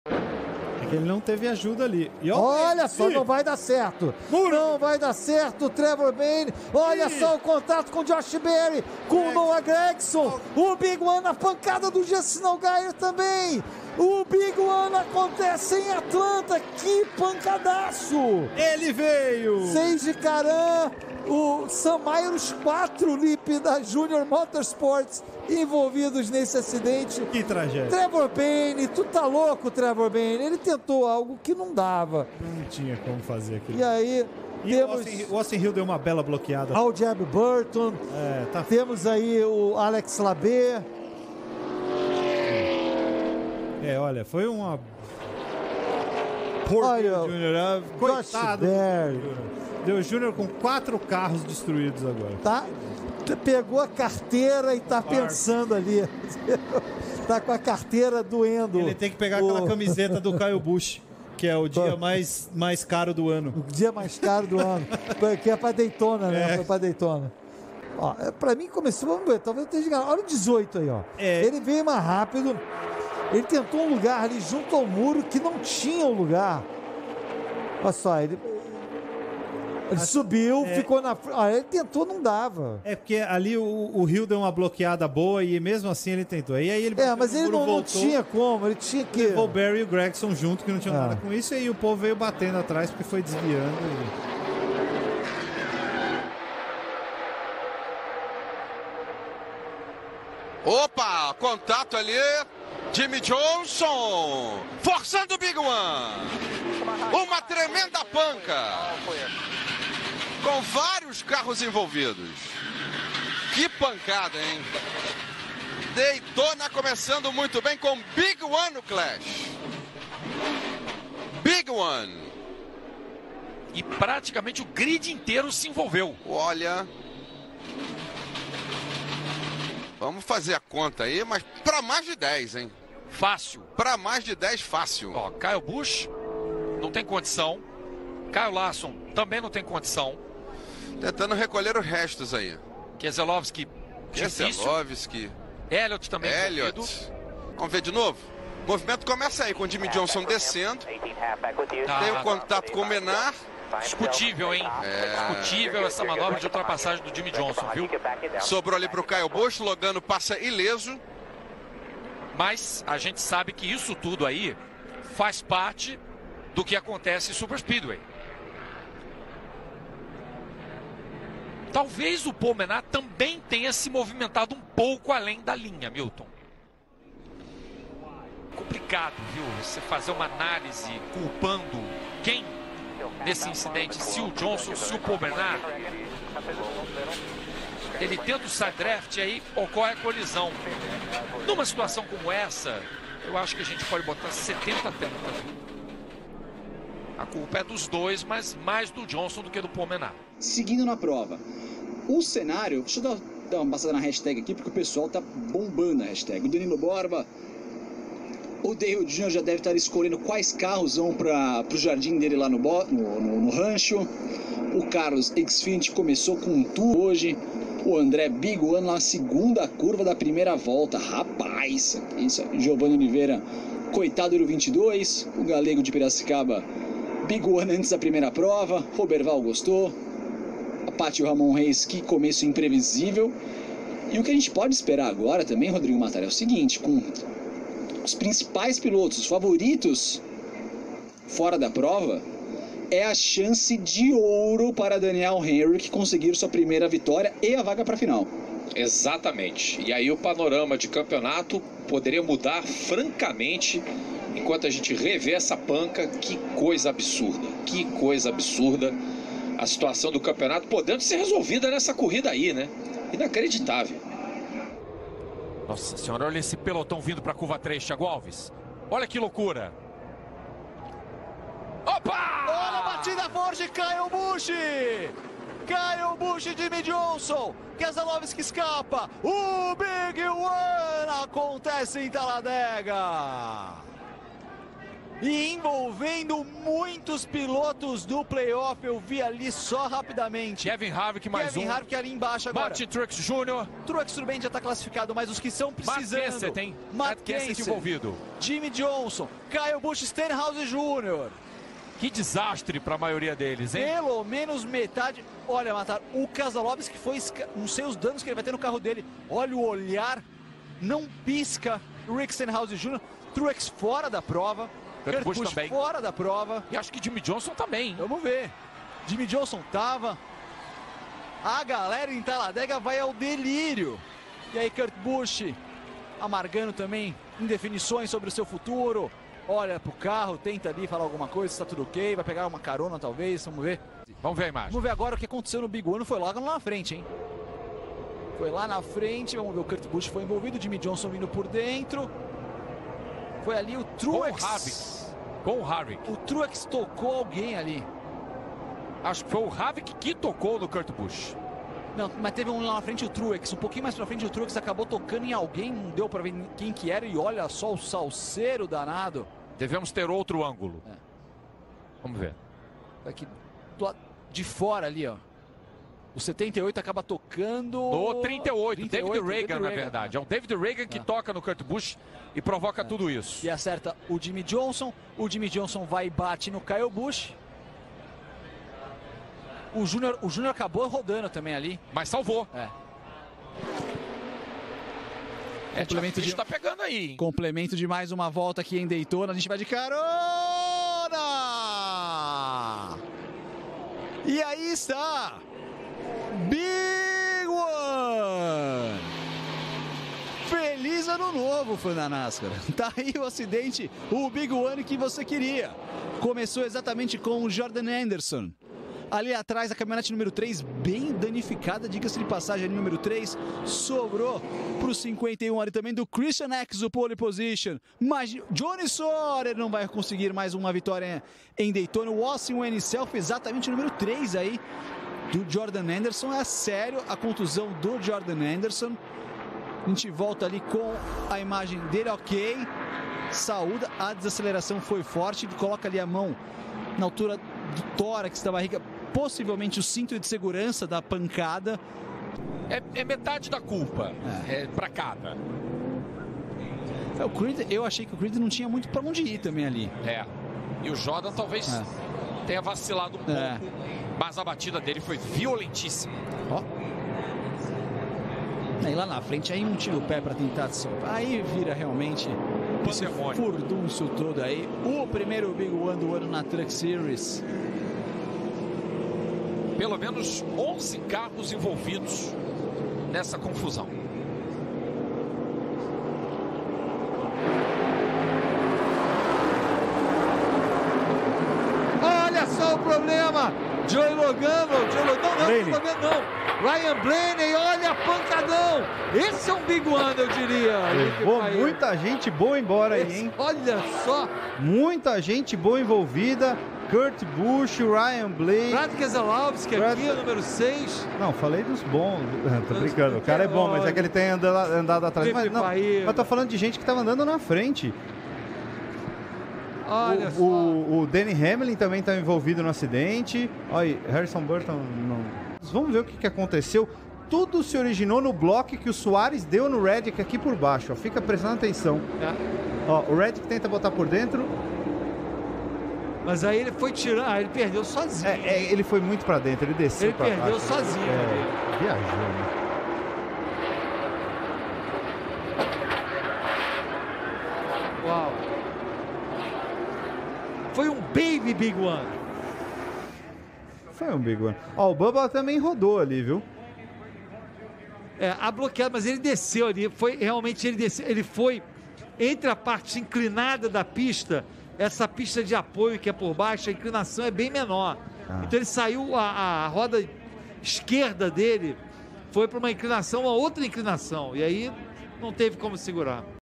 Os Melhores Big Ones da NASCAR #2
0:00 NASCAR Xfinity Series - Atlanta 2022
1:36 NASCAR Cup Series - The Clash 2019
3:19 NASCAR Xfinity Series - Kansas 2018
4:24 NASCAR Cup Series - Talladega  2017
6:23 NASCAR Truck Series - Daytona 2019
7:57 NASCAR Cup Series - Charlotte 600 2022